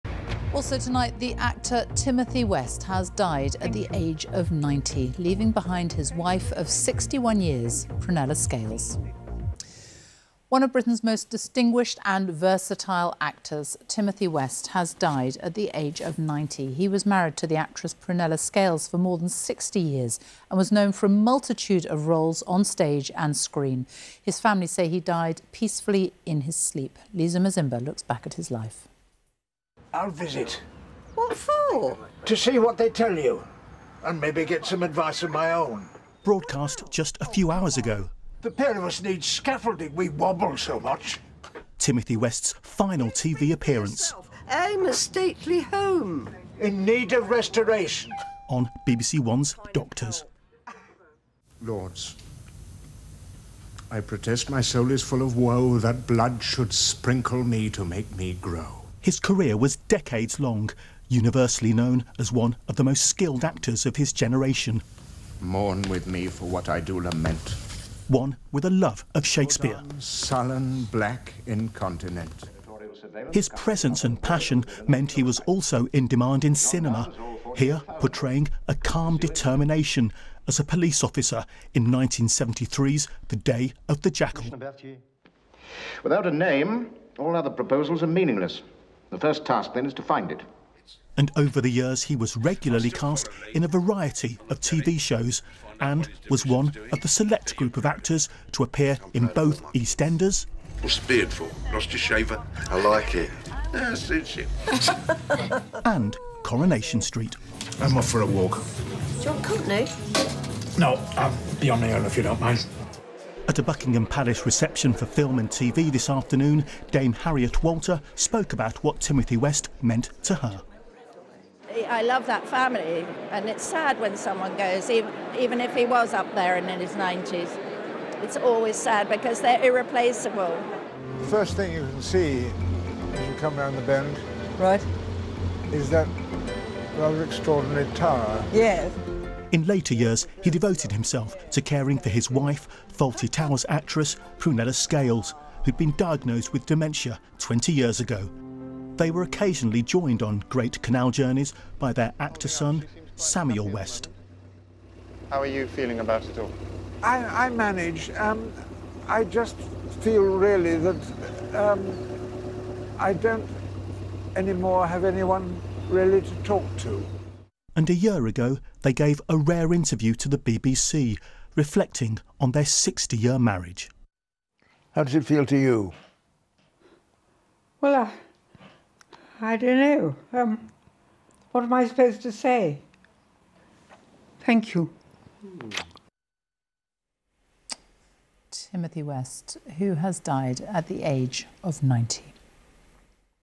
Timothy West passes away (1934 - 2024) (UK) - UK News - 13/Nov/2024
Timothy West, one of Britain's most distinguished and versatile actors, has died at the age of 90, his family have announced. He died yesterday (20 October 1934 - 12 November 2024 ).

He was known for roles on stage and screen including in TV sitcoms Not Going Out and Bedtime, dramas such as Bleak House and Gentleman Jack, and soaps Coronation Street and EastEnders.

A statement released by his children, external said the actor died "peacefully in his sleep" and was "with his friends and family at the end".

The actor is also survived by his wife, Fawlty Towers star Prunella Scales, to whom he was married for 61 years.

In recent years, the couple had been followed in 10 series of Channel 4's Great Canal Journeys.

Wikipedia:
Timothy Lancaster West, CBE was an English actor and presenter. He appeared frequently on stage and television, including stints in both Coronation Street and EastEnders, and Not Going Out, as the original Geoffrey Adams. He was married to the actress Prunella Scales.